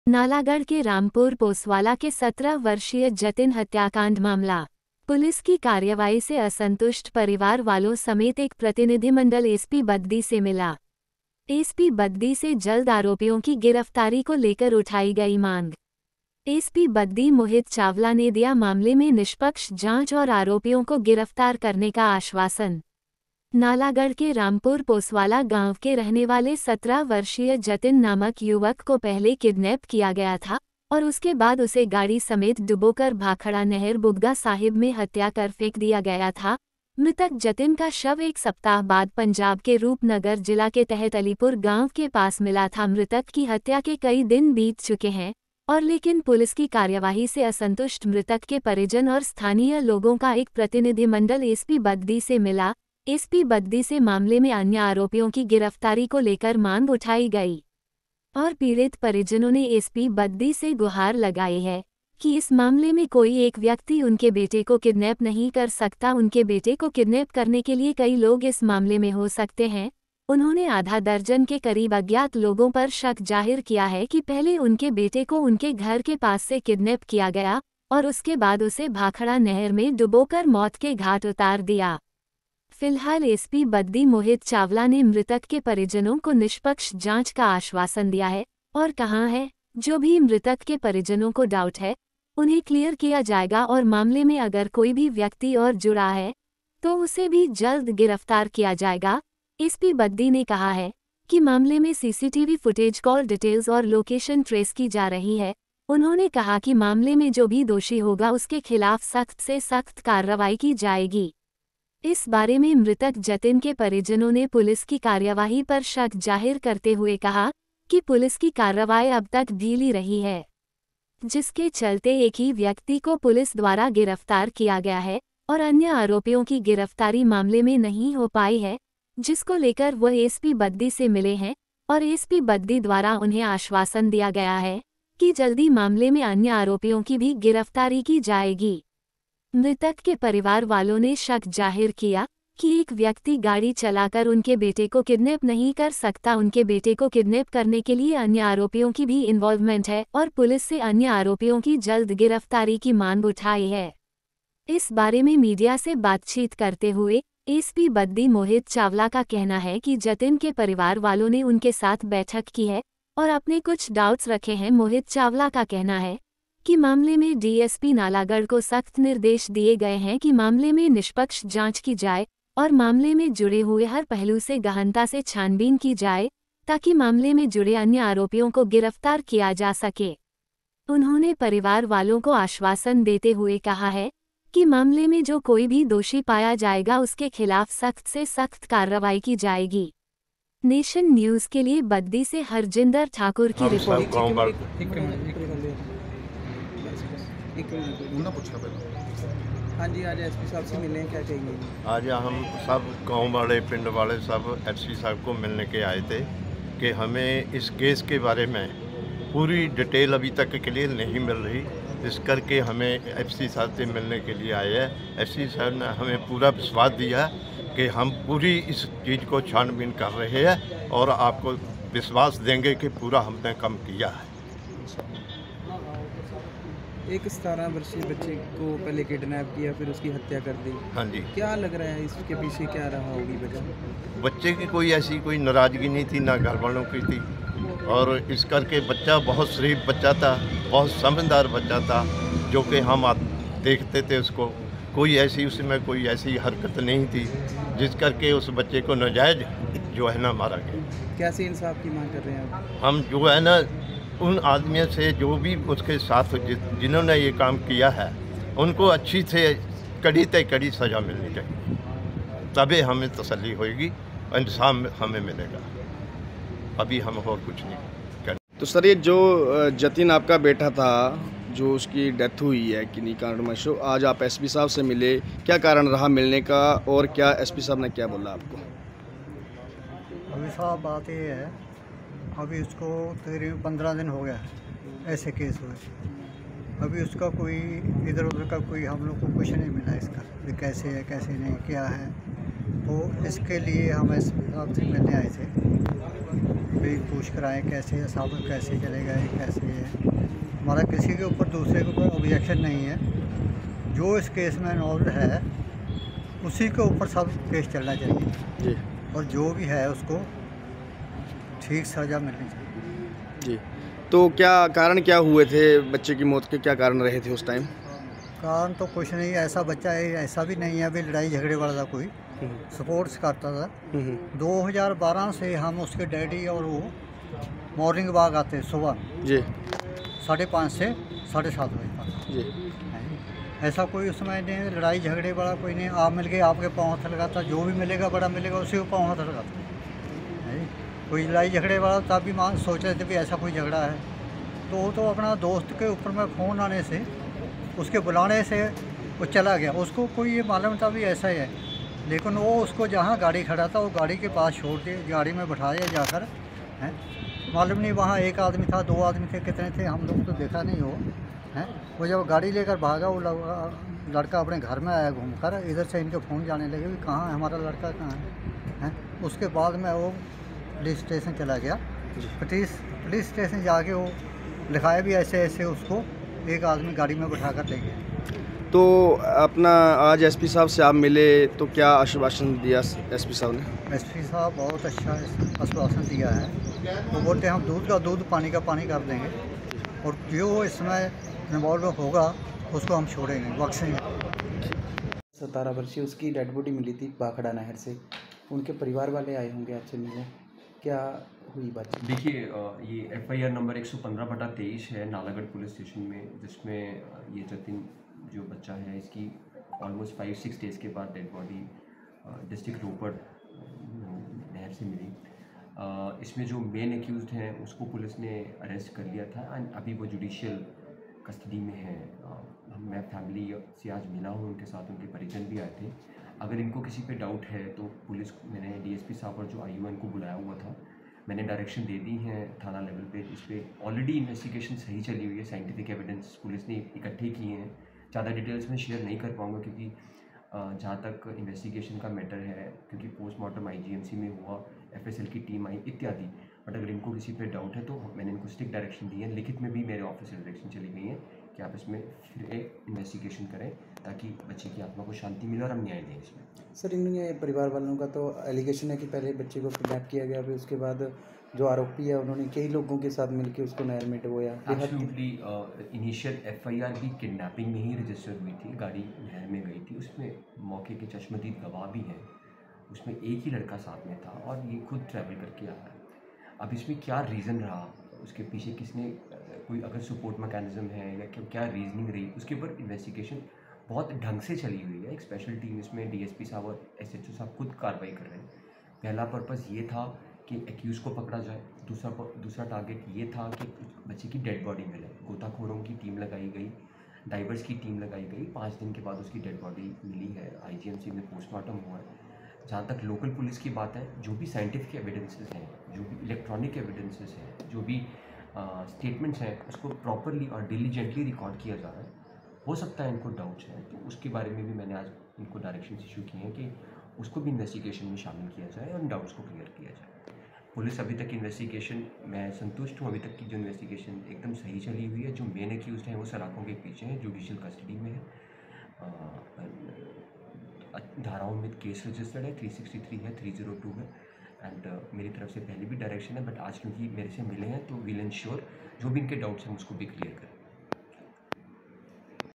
नालागढ़ के 17 वर्षीय जतिन हत्याकांड मामला पुलिस की कार्यवाई से असंतुष्ट परिवार वाले
बद्दी से हरजिन्दर ठाकुर की रिपोर्ट:-

नालागढ़ के रामपुर पोसवाला  के 17 वर्षीय जतिन हत्याकांड मामला
पुलिस की कार्यवाई से असंतुष्ट परिवार वालों समेत  एक प्रतिनिधिमंडल एसपी बद्दी से मिला
एसपी बद्दी से जल्द आरोपियों की गिरफ्तारी को लेकर उठाई गई मांग
एसपी बद्दी मोहित चावला ने दिया मामले में निष्पक्ष जांच और आरोपियों को गिरफ्तार करने का आश्वासन

एंकर : नालागढ़ के रामपुर पोसवाला गांव के रहने वाले 17 वर्षीय जतिन नामक युवक को पहले किडनैप किया गया था और उसके बाद उसे गाड़ी समेत डुबोकर भाखड़ा नहर बुग्गा साहिब में हत्या कर फेंक दिया गया था मृतक जतिन का शव 1 सप्ताह बाद पंजाब के रूप नगर जिला के तहत अलीपुर गांव के पास मिला था मृतक की हत्या के कई दिन बीत चुके हैं और लेकिन पुलिस की कार्यवाही से असंतुष्ट मृतक के परिजन और स्थानीय लोगों का एक प्रतिनिधिमंडल एसपी बद्दी से मिला एसपी बद्दी से मामले में अन्य आरोपियों की गिरफ्तारी को लेकर मांग उठाई गई।

और पीड़ित परिजनों ने एसपी बद्दी से गुहार लगाई है कि इस मामले में कोई एक व्यक्ति उनके बेटे को किडनैप नहीं कर सकता उनके बेटे को किडनैप करने के लिए कई लोग इस मामले में हो सकते हैं उन्होंने आधा दर्जन के करीब अज्ञात लोगों पर शक जाहिर किया है कि पहले उनके बेटे को उनके घर के पास से किडनैप किया गया और उसके बाद उसे भाखड़ा नहर में डुबोकर मौत के घाट उतार दिया फिलहाल एसपी बद्दी मोहित चावला ने मृतक के परिजनों को निष्पक्ष जांच का आश्वासन दिया है और कहां है जो भी मृतक के परिजनों को डाउट है उन्हें क्लियर किया जाएगा और मामले में अगर कोई भी व्यक्ति और जुड़ा है तो उसे भी जल्द गिरफ्तार किया जाएगा एसपी बद्दी ने कहा है कि मामले में सीसीटीवी फुटेज कॉल डिटेल्स और लोकेशन ट्रेस की जा रही है उन्होंने कहा कि मामले में जो भी दोषी होगा उसके खिलाफ सख्त से सख्त कार्रवाई की जाएगी।

 इस बारे में मृतक जतिन के परिजनों ने पुलिस की कार्यवाही पर शक जाहिर करते हुए कहा कि पुलिस की कार्रवाई अब तक ढीली रही है जिसके चलते एक ही व्यक्ति को पुलिस द्वारा गिरफ्तार किया गया है और अन्य आरोपियों की गिरफ्तारी मामले में नहीं हो पाई है जिसको लेकर वह एसपी बद्दी से मिले हैं और एसपी बद्दी द्वारा उन्हें आश्वासन दिया गया है कि जल्दी मामले में अन्य आरोपियों की भी गिरफ्तारी की जाएगी। मृतक के परिवार वालों ने शक जाहिर किया कि एक व्यक्ति गाड़ी चला कर उनके बेटे को किडनैप नहीं कर सकता उनके बेटे को किडनैप करने के लिए अन्य आरोपियों की भी इंवॉल्वमेंट है और  पुलिस से अन्य आरोपियों की जल्द गिरफ्तारी की मांग उठाई है।

इस बारे में मीडिया से बातचीत करते हुए एसपी बद्दी मोहित चावला का कहना है कि जतिन के परिवार वालों ने उनके साथ बैठक की है और अपने कुछ डाउट्स रखे हैं मोहित चावला का कहना है कि मामले में डीएसपी नालागढ़ को सख्त निर्देश दिए गए हैं कि मामले में निष्पक्ष जांच की जाए और मामले में जुड़े हुए हर पहलू से गहनता से छानबीन की जाए ताकि मामले में जुड़े अन्य आरोपियों को गिरफ्तार किया जा सके उन्होंने परिवार वालों को आश्वासन देते हुए कहा है कि मामले में जो कोई भी दोषी पाया जाएगा उसके खिलाफ सख्त से सख्त कार्रवाई की जाएगी।